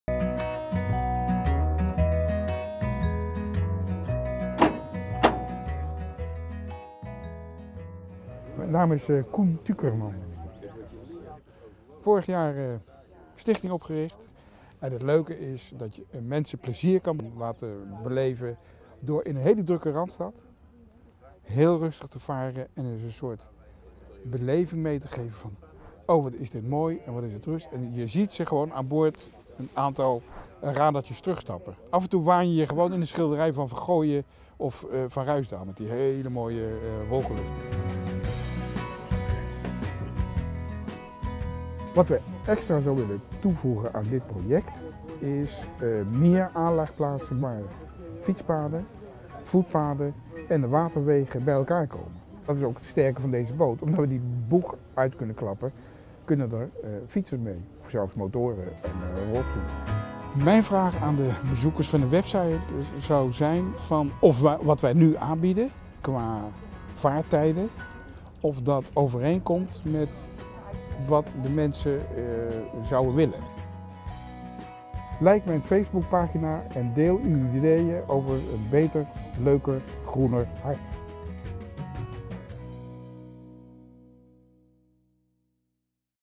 ANWB - Zoetermeervaart
De ANWB heeft recreatie hoog in het vaandel staan. En heeft dit initiatief in het leven geroepen om ervaring op te doen hoe je met bewoners, bezoekers/leden, ondernemers en betrokken overheden succesvol kunt samenwerken om het Groene Hart West aantrekkelijker te maken. Hier vind je 7 nieuwe recreatieplannen van ondernemers. Een van deze ideeën wordt gerealiseerd met steun van de ANWB. Welk idee dat wordt? Dat bepaal jij. Denk mee en geef je mening!